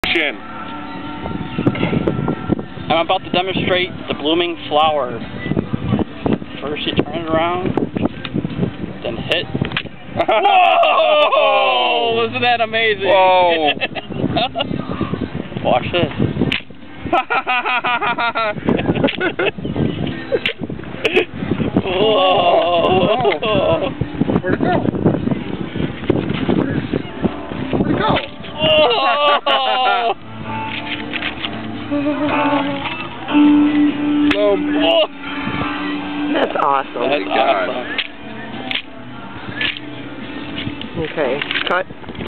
Double A demonstrates the Blooming Flower
Double A demonstrates the amazing Blooming Flower

Credits:
Director: Vicki
Producer: Vicki
Cameraman: Vicki
Gaffer: Vicki

STARRING: Double A
CoStarring: Skip